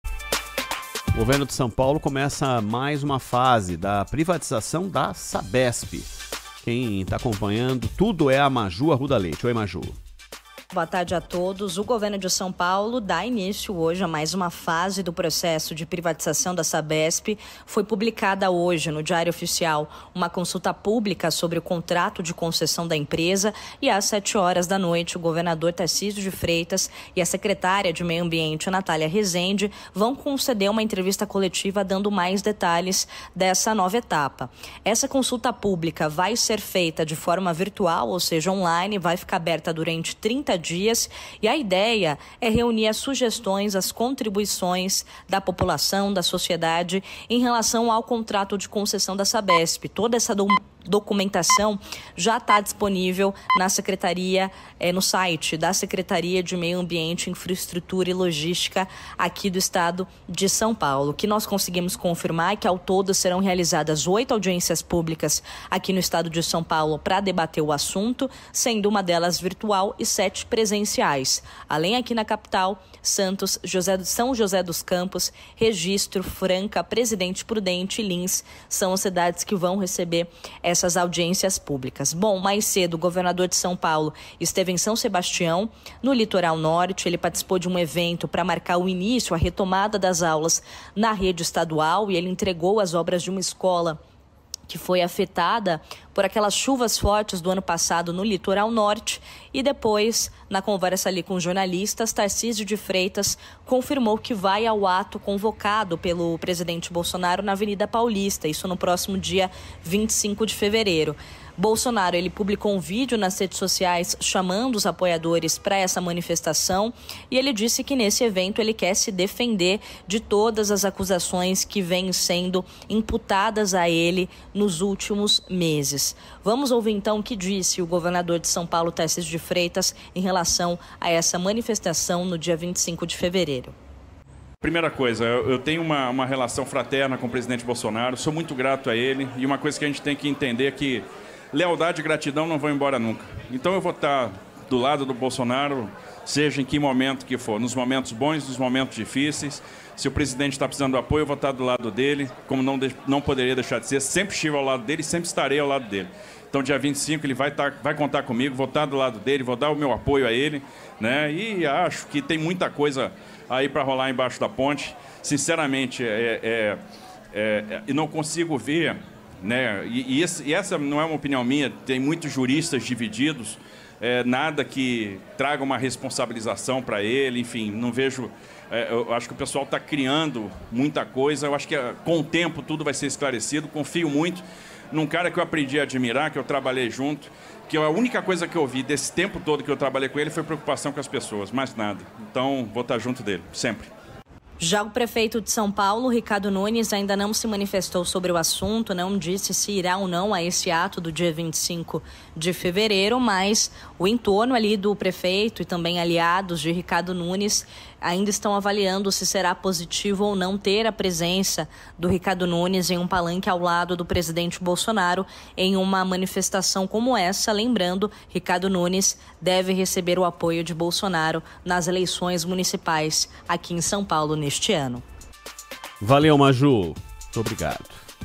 Governo de SP inicia mais uma fase da privatização da Sabesp
O governo de São Paulo divulgou, nesta quinta-feira (15), o início de mais uma fase para a privatização da Sabesp.

Mais informações com a repórter Maju Arruda Leite.